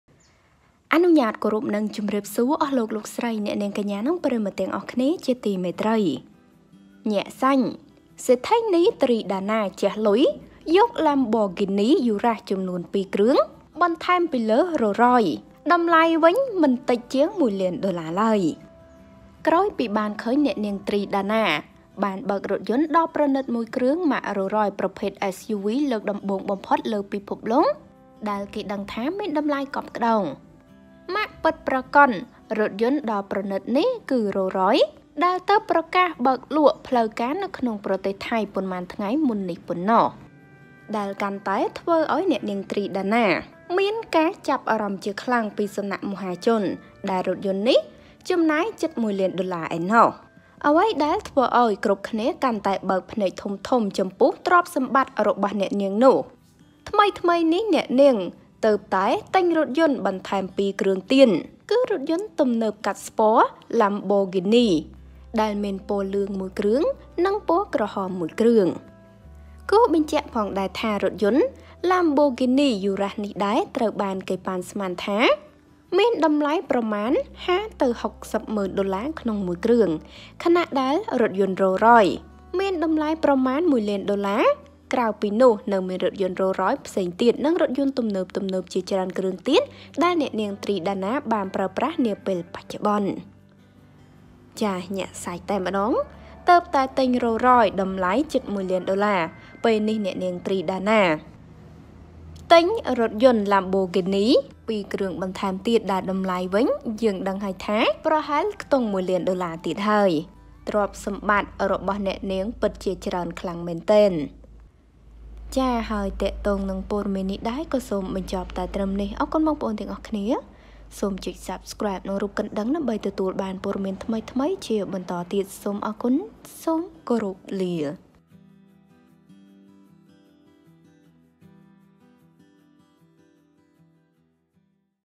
សេដ្ឋីនី ទ្រី ដាណា ជះលុយយក Lamborghini Urus ចំនួន 2គ្រឿងទៀតមានតំលៃរហូតដល់…
ក្រោយ​ពី​គេ​បាន​ឃើញ​អ្នកនាង ទ្រី ដាណា បាន​បើករថយន្ត​ដ៏​ប្រណិត​មួយ​ ម៉ាក Roll Royce ប្រភេទ SUV លើក​ដំបូង​បំផុត​លើ​ពិភព​លោក ដែល​គេ​ដឹង​ថា​មាន​តម្លៃ​កប់​ក្ដោង។ម៉ាក​ពិត​ប្រាកដ​រថយន្ត​ដ៏​ប្រណិត​នេះ គឺ Rolls-Royce Cullinan ដែល​ទើប​ប្រកាស​បើក​លក់​ផ្លូវ​ការ​នៅ​ក្នុង​ប្រទេស​ថៃ​ប៉ុន្មាន​ថ្ងៃ​មុន​នេះ​ប៉ុណ្ណោះ។ឥឡូវនេះកាន់តែភ្ញាក់ផ្អើលទៀតហើយ អ្នកនាង ទ្រី ដាណា បានចំណាយលុយជាង 1លានដុល្លារបន្ថែមទៀត ដើម្បី ទិញរថយន្តទំនើបកាត់ស្ព័រ Lamborghini Urus ចំនួន2គ្រឿងបន្ថែមទៀត ដែលមានពណ៌លឿងមួយគ្រឿង និងពណ៌ក្រហម មួយគ្រឿង។

សូមអធ្យាស្រ័យ

ចំពោះការមិនបានតប ខំមិនបានគ្រប់គ្នា

Ms Socheata